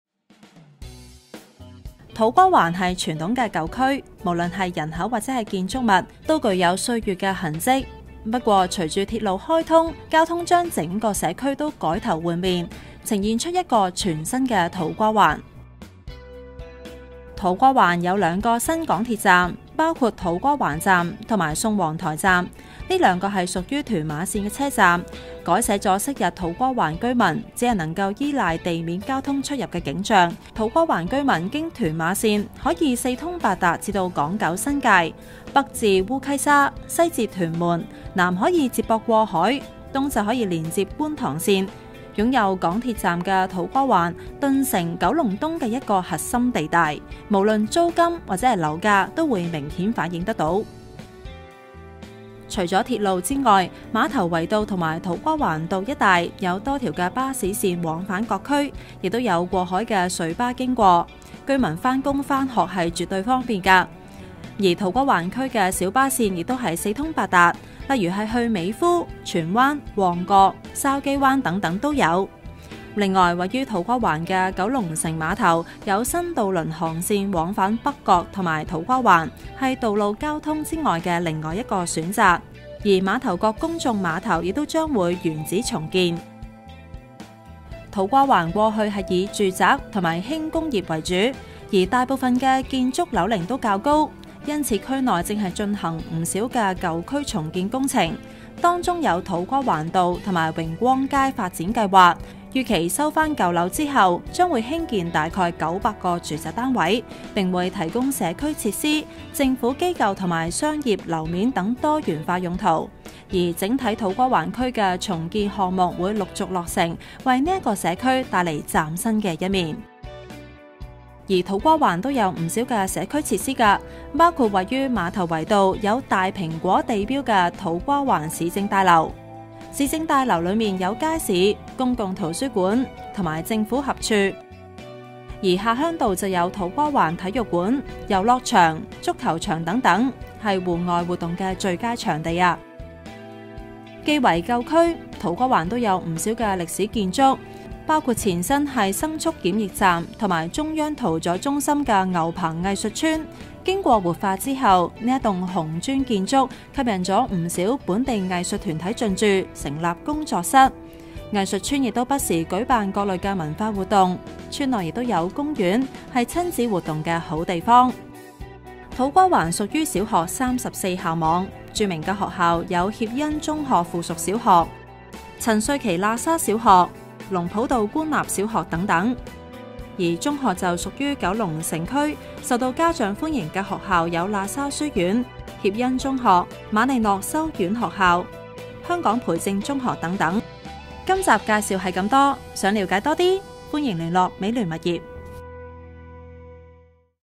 【土瓜灣】雙鐵路站優勢 嶄新社區出現
土瓜灣是傳統的舊區，
無論是人口或建築物，
均具有歲月的痕跡，
不過，隨著鐵路開通，
交通將整個社區改頭換面，
呈現出一個全新的土瓜灣。

土瓜灣將有兩個新的港鐵站，
包括土瓜灣站及宋皇臺站，
這兩個屬於屯馬線的車站，
改寫昔日土瓜灣居民，只能依賴地面交通出入的景象，
亦一解居民長期飽受塞車之苦。
土瓜灣居民經屯馬線，可以四通八達至港九新界，
北至烏溪沙，西至屯門，南可以接駁過海，東可連接觀塘線，
擁有港鐵站的土瓜灣，頓成九龍東的一個核心地帶，
無論租金或樓價都會明顯反映。

除了鐵路之外，馬頭圍道及土瓜灣道一帶，
有多條巴士線往返各區，亦有過海隧巴經過，
居民返工返學絕對方便。
而土瓜灣區的小巴線亦四通八達，
例如往美孚、荃灣、旺角及筲箕灣等都有。

另外，位於土瓜灣的九龍城碼頭，
有新渡輪航線往返北角與土瓜灣，
是道路交通外的另一個選擇。
而馬頭角公眾碼頭亦將會原址重建。

土瓜灣過去以住宅及輕工業為主，
而大部分建築樓齡較高，
因此區內正進行不少舊區重建工程，
其中有土瓜灣道及榮光街發展計畫，
預期收回舊樓後，將興建約900個住宅單位，
並會提供社區設施、政府機構及商業樓面等多元化用途，
重建項目並會加建天橋，橫跨土瓜灣道，連接土瓜灣港鐵站，
而整體土瓜灣區的重建項目會陸續落成，
為這個社區帶來嶄新的一面。

而土瓜灣有不少社區設施，
包括位於馬頭圍道、有「大蘋果」地標的土瓜灣市政大樓，
街坊的民生必需品，通通都能夠找到，
市政大樓內有街市、公共圖書館及政府合署，住這裡的街坊一定知。
而下鄉道有土瓜灣體育館、遊樂場、足球場等，
是戶外活動最佳場地。

既為舊區，土瓜灣有不少歷史建築，
包括前身為牲畜檢疫站及中央屠宰中心的牛棚藝術村，
經過活化後，這幢紅磚建築，
吸引不少本地藝術團體進駐，成立工作室，
藝術村亦不時舉辦各類文化活動，
村內亦有公園，是親子活動的好地方。

其實土瓜灣區有不少街坊小店，閑閑地都幾十年歷史，
例如位於安徽街的餃子老店、
於土瓜灣無人不識、位於落山道的排骨飯、
又或者老牌糖水檔等，都充滿社區色彩，相當受街坊歡迎。

土瓜灣屬於小學34校網，著名的學校有協恩中學附屬小學、
聖羅撒學校、保良局何壽南小學及陳瑞祺（喇沙）小學等。
而中學屬九龍城區，受家長歡迎的學校有喇沙書院、協恩中學、
瑪利諾修院學校、拔萃男書院、香港培正中學等。

今集介紹係咁多，想了解更多？歡迎聯絡美聯物業。